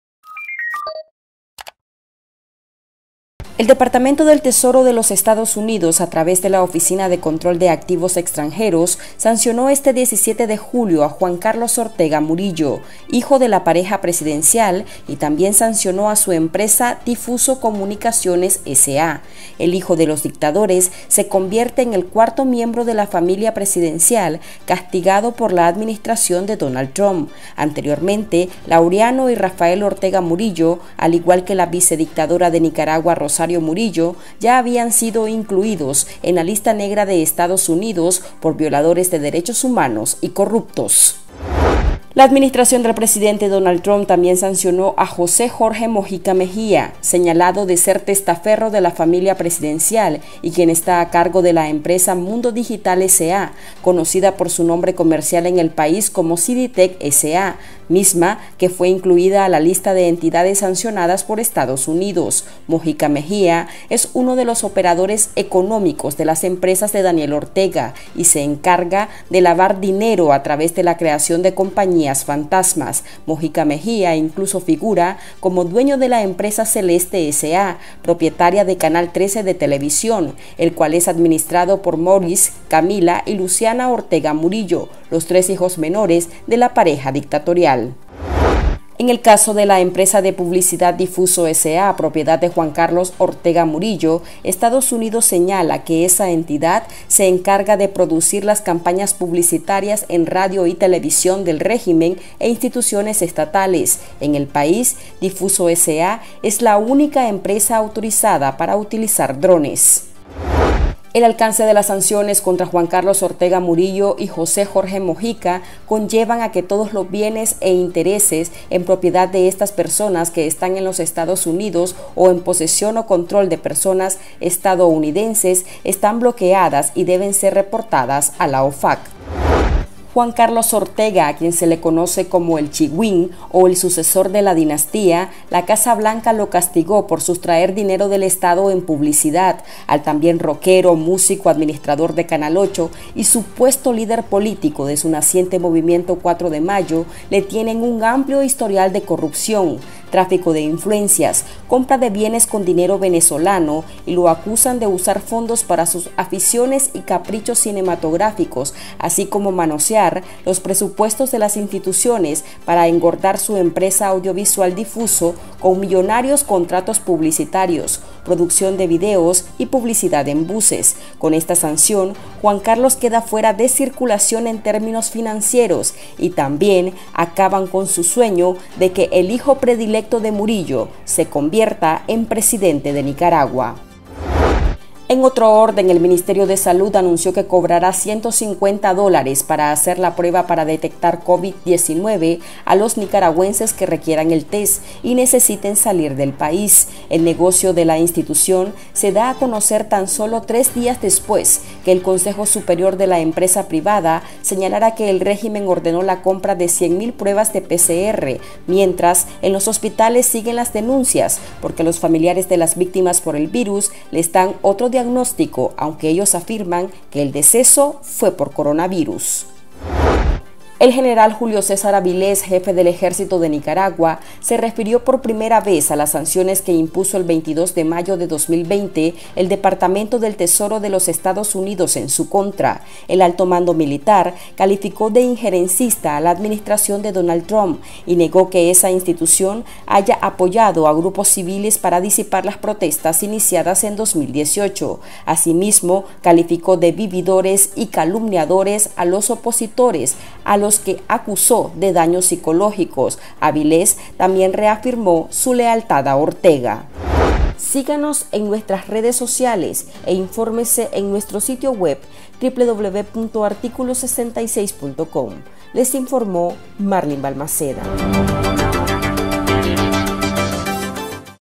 Este es el historial delictivo de Juan Carlos Ortega Murillo, sancionado por Estados Unidos
Corrupción, lavado de dinero, operaciones delictivas para sustraer dinero público y enriquecimiento a través de su empresa audiovisual, con la que produce la propaganda gubernamental, son parte de las acusaciones del Departamento del Tesoro norteamericano contra el hijo predilecto de Rosario Murillo.